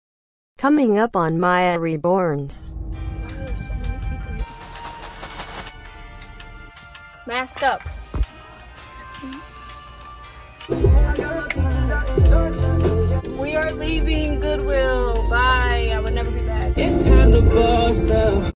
Reborn Outing Series Trailer! | Mya Reborns
Hey everyone , I decided to put together a quick video on what you should expect for my next upcoming videos! I thought it would be super fun to give you all a preview before the video comes out!